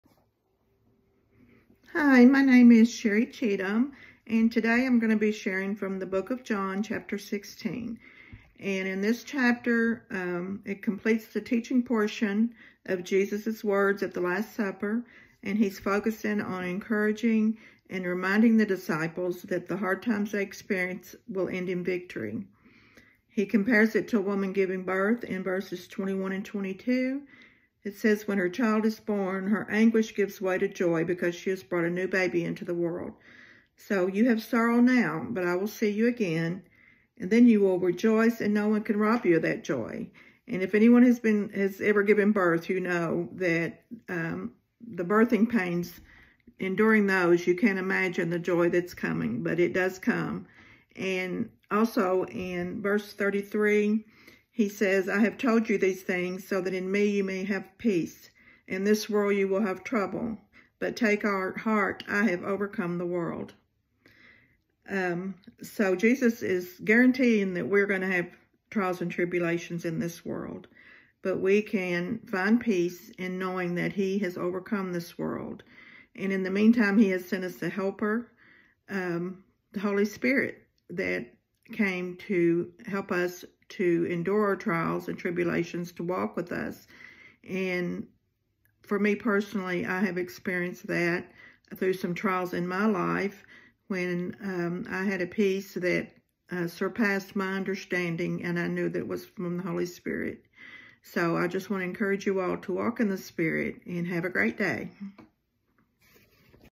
Lent DAY 36 | March 26 | John 16
Sherry Cheatham offers the main topics in this chapter and then brings focus to the truth that Jesus has overcome the world and that the Helper, the Holy Spirit, will support us in trials.